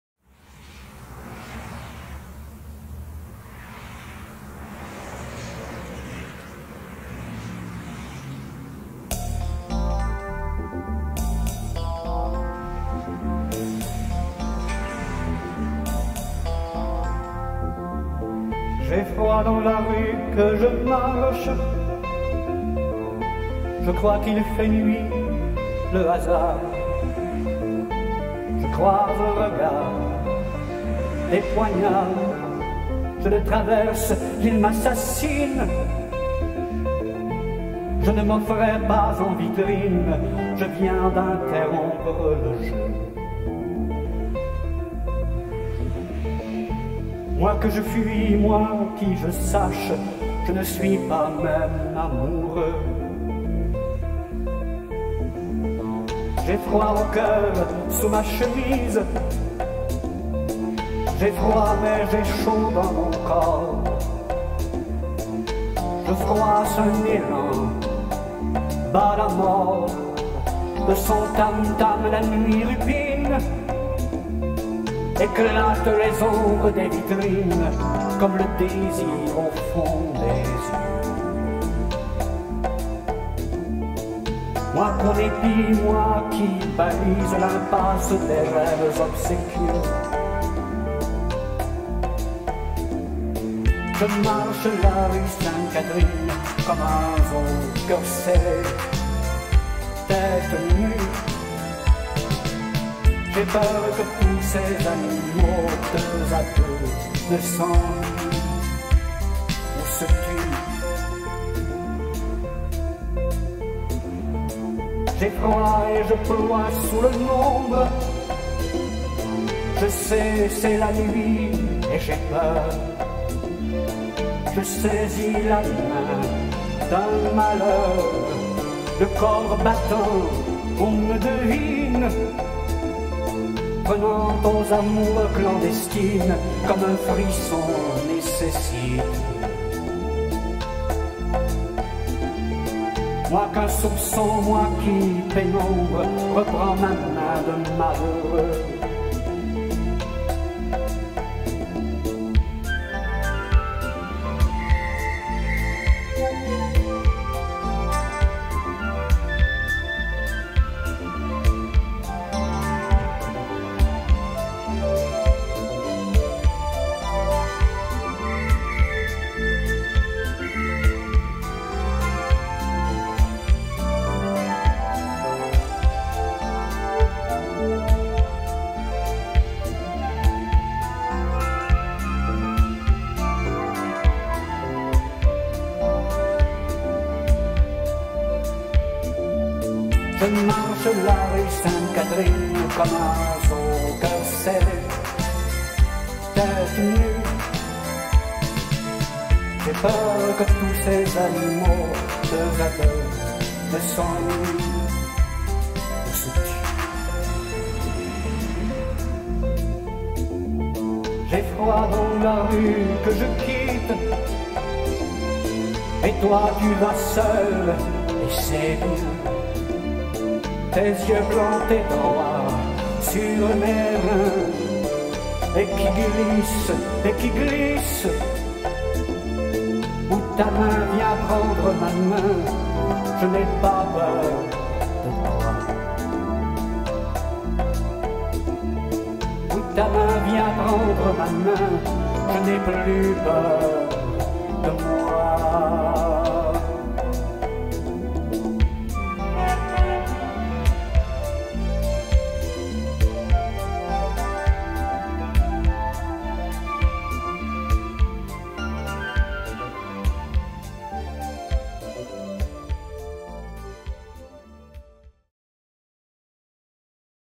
Zoo Sainte-Catherine
La quatrième, eh oui, déjà la quatrième. Marcher rue Sainte-Catherine en hiver, le temps d'une rencontre fortuite. Les sorties nocturnes qui n'alaient pas toujours bien. Enfin le froid...